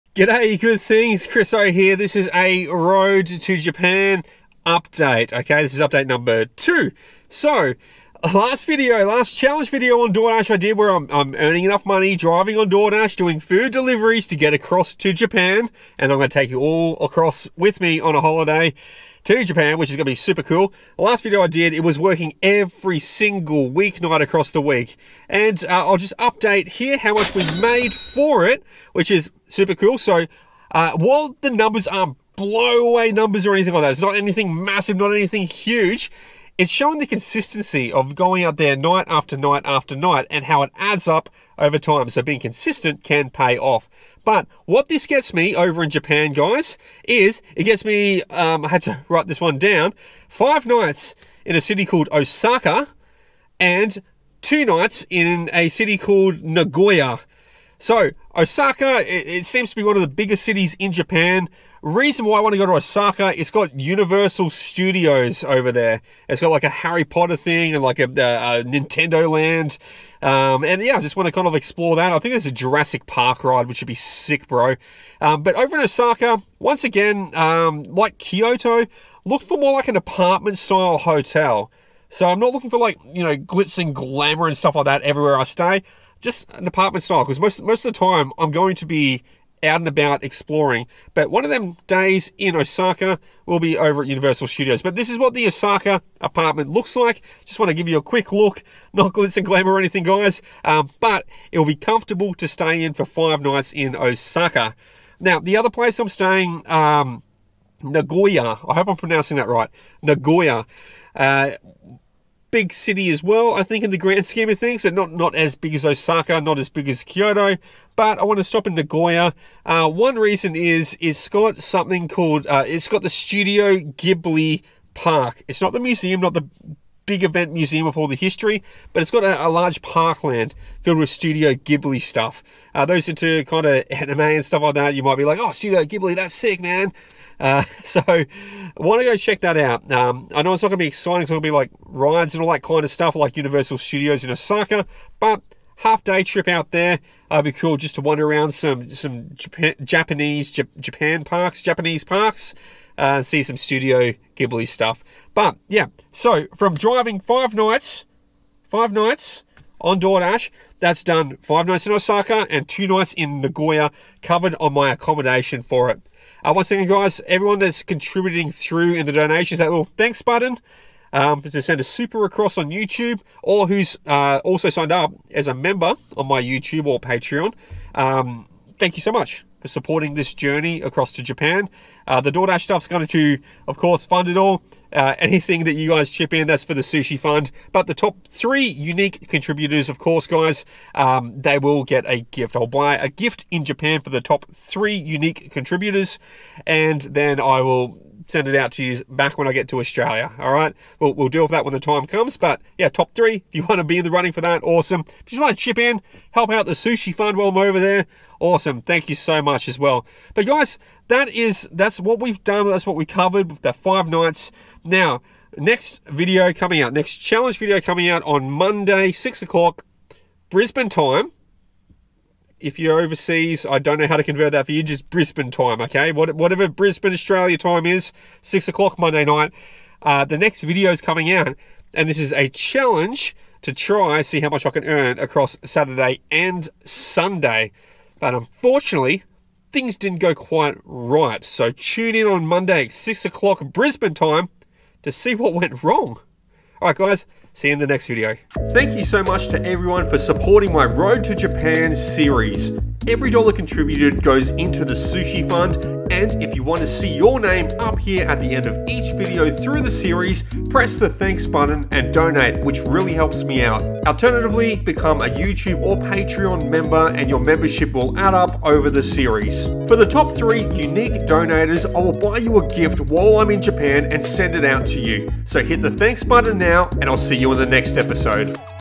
Im Doordash Driving To Japan: Update 2
Road To Japan Update 2

Thanks in advance :)
Start earning extra income with DoorDash NOW! Use my personal referral to begin your own journey and start making a change.
My partner Brooke is an artist, and I've set up a small clothing store to share her art with the world!